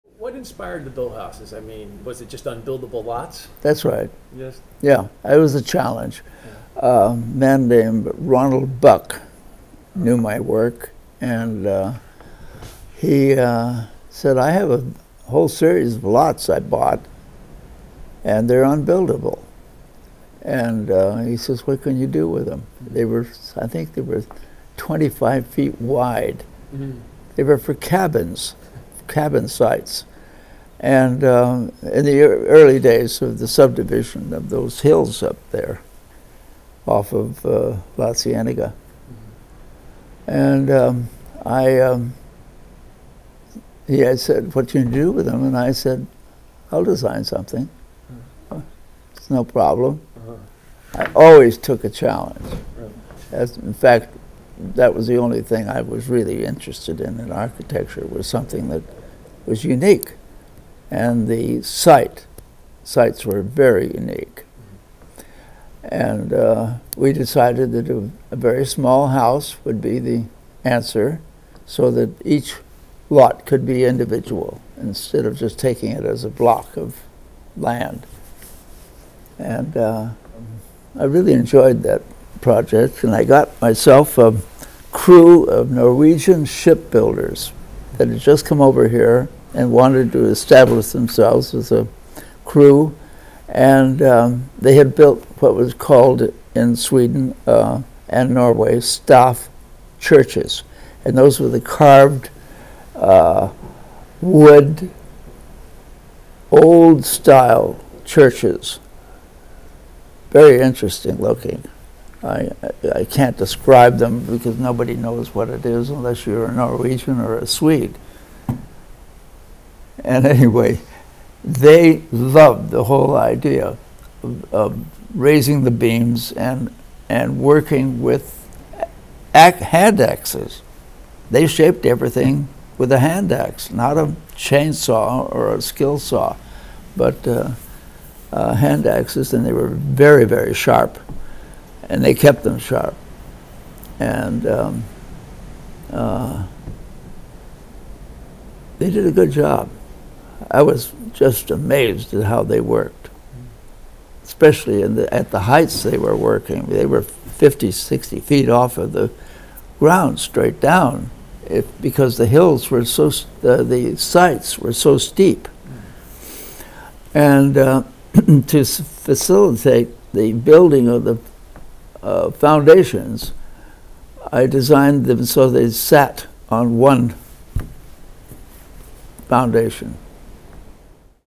Harry Gesner - Part 1
Watch as renowned architect Harry Gesner discusses the building of his famous Hollywood Hills "Boathouses"